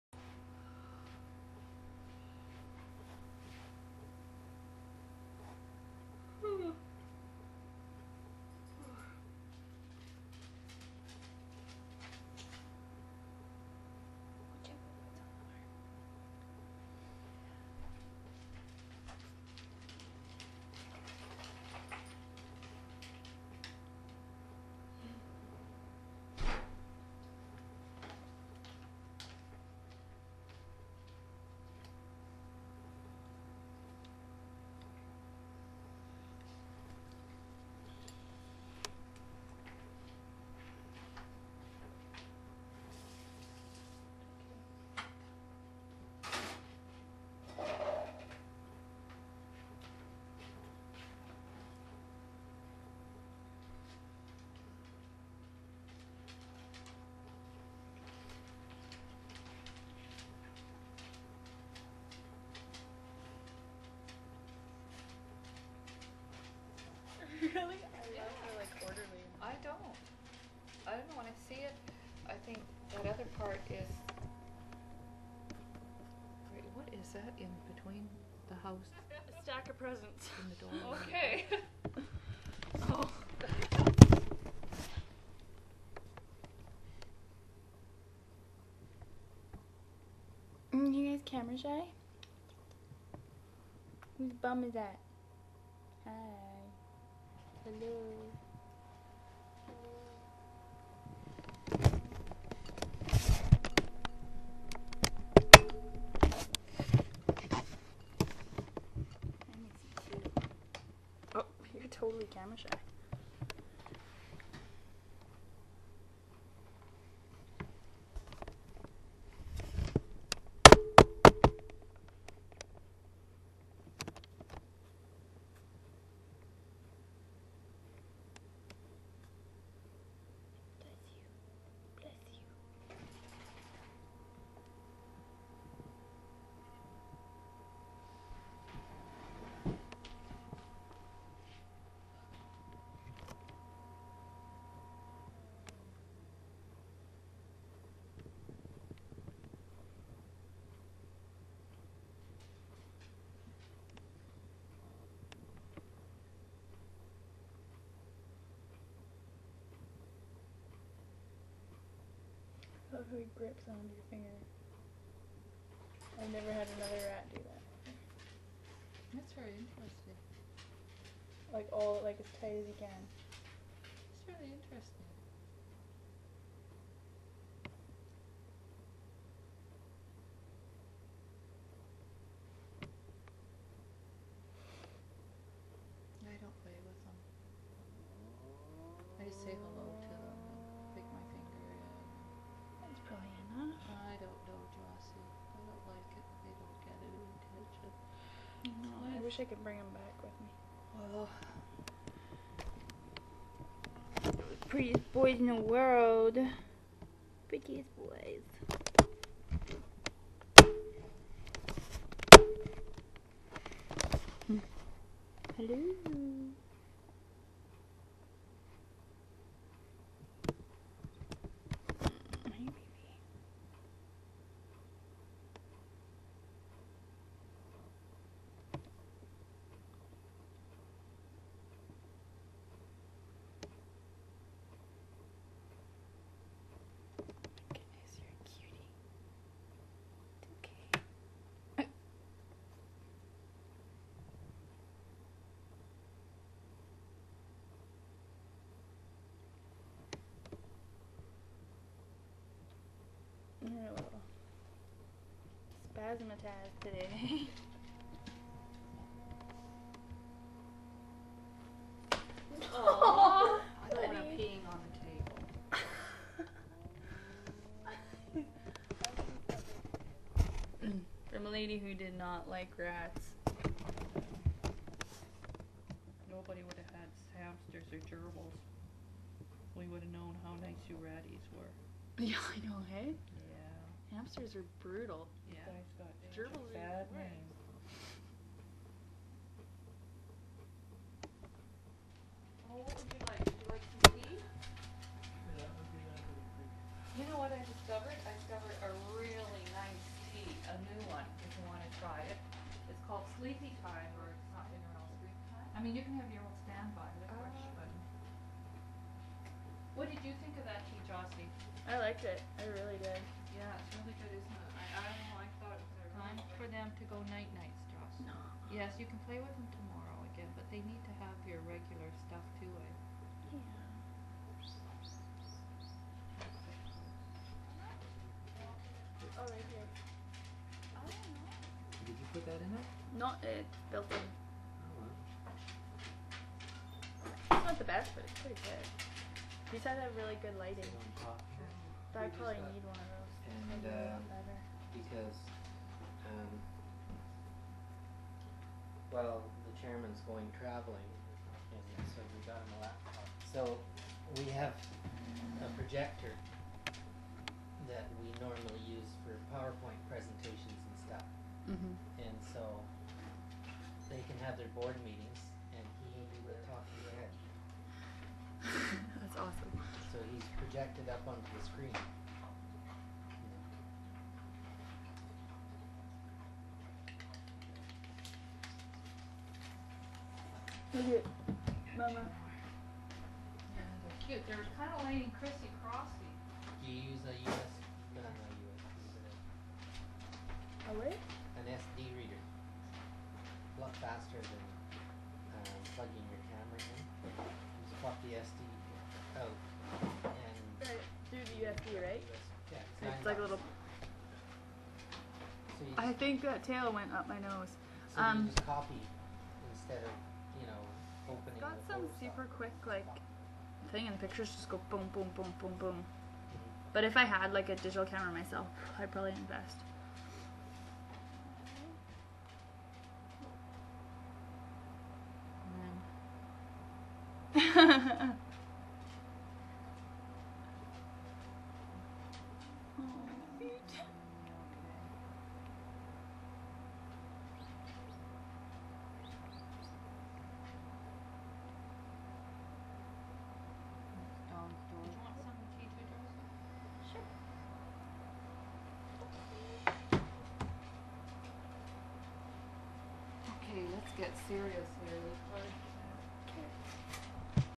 Ratties!
My babies!
I am a huge promoter of rats as pets.

Save these priceless little sparks of love and energy from becoming snake food and adopt a rat today from your local pet store or animal shelter!

I have owned over 6, and have never been bitten [which is more than I can say for ANY SINGLE gerbil or hamster I have owned].

They are quiet (most of the time)
And very clean!
They are also super-smart.
I call them my apartment dogs... but I don't have to walk them.

Amazing animals, these rats.

Black one is Ranger and Black and White one is Sport :)